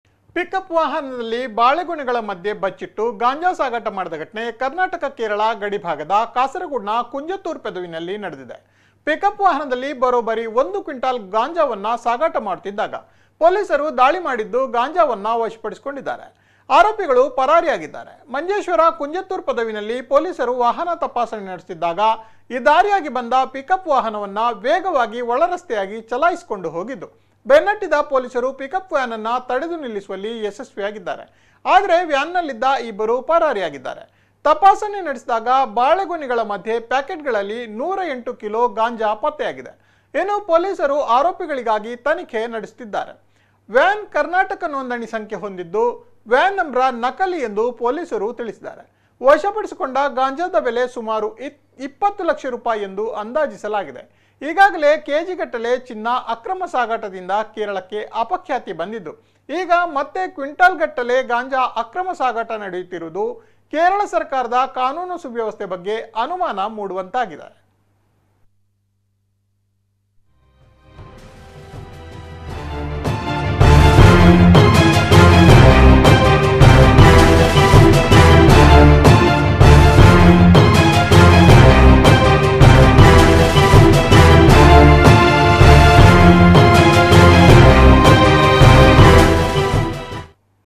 ಕಾಸರಗೋಡು ಗಡಿಯಲ್ಲಿ ಸಿಕ್ಕಿದ್ದು ಬರೋಬ್ಬರಿ 1 ಕ್ವಿಂಟಾಲ್ ಗಾಂಜಾ....!!
ಪಿಕಪ್ ವಾಹನದಲ್ಲಿ ಬಾಳೆಗೊನೆಗಳ ಮಧ್ಯೆ ಬಚ್ಚಿಟ್ಟು ಗಾಂಜಾ ಸಾಗಾಟ ಮಾಡಿದ ಘಟನೆ ಕರ್ನಾಟಕ ಕೇರಳ ಗಡಿಭಾಗದ ಕಾಸರಗೋಡಿನ ಕುಂಜತ್ತೂರು ಪದವಿನಲ್ಲಿ ನಡೆದಿದೆ. ಪಿಕಪ್ ವಾಹನದಲ್ಲಿ ಬರೋಬ್ಬರಿ 1 ಕ್ವಿಂಟಾಲ್ ಗಾಂಜಾವನ್ನು ಸಾಗಾಟ ಮಾಡುತ್ತಿದ್ದಾಗ ಪೋಲಿಸರು ದಾಳಿ ಮಾಡಿದ್ದು, ಗಾಂಜಾ ವಶಪಡಿಸಿಕೊಂಡಿದ್ದಾರೆ. ಆರೋಪಿಗಳು ಪರಾರಿಯಾಗಿದ್ದಾರೆ. ಮಂಜೇಶ್ವರ ಕುಂಜತ್ತೂರು ಪದವಿನಲ್ಲಿ ಪೊಲೀಸರು ವಾಹನ ತಪಾಸಣೆ ನಡೆಸುತ್ತಿದ್ದಾಗ, ಈ ದಾರಿಯಾಗಿ ಬಂದ ಪಿಕಪ್ ವ್ಯಾನ್‌ನ್ನು ವೇಗವಾಗಿ ಒಳ ರಸ್ತೆಯಾಗಿ ಚಲಾಯಿಸಿಕೊಂಡು ಹೋಗಿದ್ದು